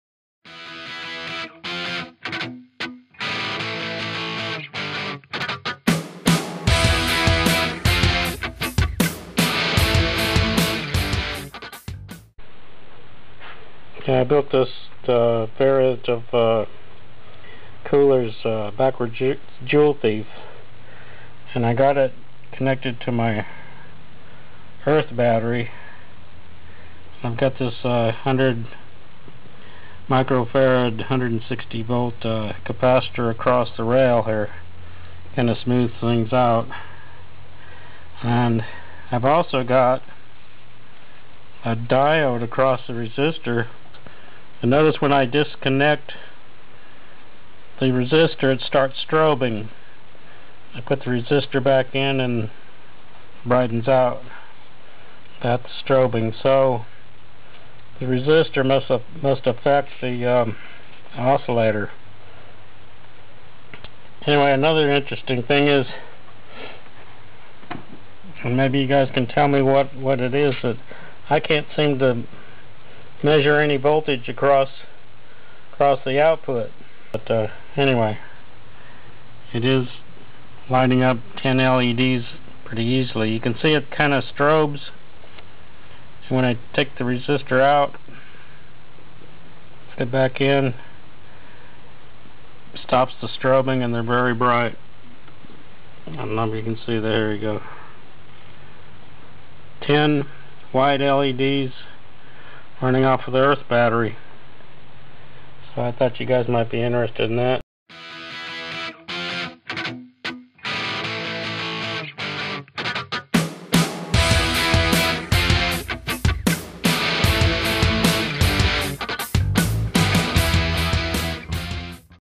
Modded Kooler's Backwards Joule Thief Connected to Earth Battery
This video shows a modified Kooler's Backwards Joule Thief connected to my earth battery cells.  The earth battery is composed of three dipole magnesium ribbon/copper long wire cells outputting about 1.6 V DC and about 6-10 milliamps.  When connected to a basic joule thief it lights 1 or 2 3 mm red leds.

With Kooler's modifed Backwards Joule Thief it can light 10 5 mm bright white leds.

My mod includes a diode across the resistor effectively lowering the emf through the resistor.  I didn't have a 470K ohm pot so I used a 120K ohm resistor and the diode.  I may be pushing too much current through the 2N3904 but it doesn't seem to be getting hot.  I also wound about 30 turns on the secondary vs 15 turns shown in Kooler's schematic.

For some reason I can't measure the voltage at the output with a meter.  It is obvious there is enough emf to light 10 leds.  I can see the voltage on the scope but I just got it and don't know how to read it (analog).  I need to study the manual...